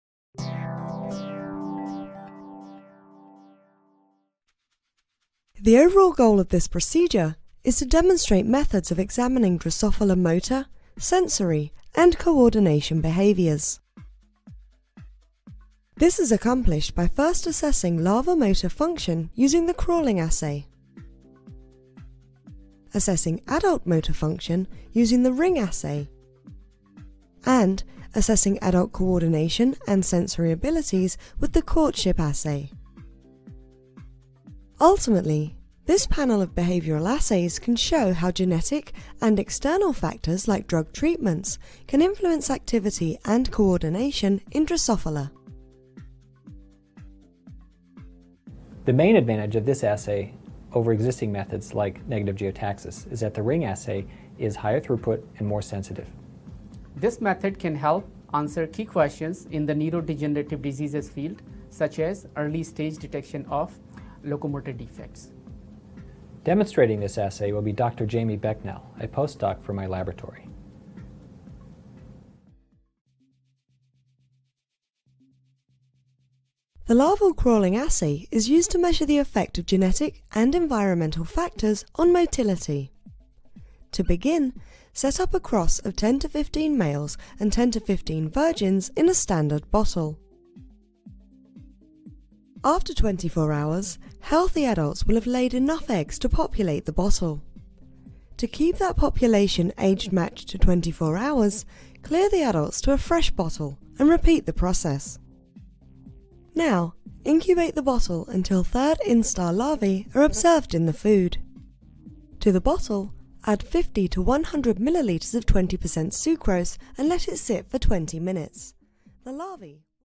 Methods To Assay Drosophila Behavior l Protocol Preview
Methods to Assay Drosophila Behavior -  a 2 minute Preview of the Experimental Protocol

Charles D. Nichols, Jaime Becnel, Udai B. Pandey
Louisiana State University Health Sciences Center, Department of Pharmacology and Experimental Therapeutics; Louisiana State University Health Sciences Center, Department of Genetics;

Drosophila melanogaster is a genetically and behaviorally tractable model system that has been used to understand the molecular and cellular basis of many important biological processes for over a century 1. Drosophila has been well exploited to gain insights into the genetic basis of fly behavior.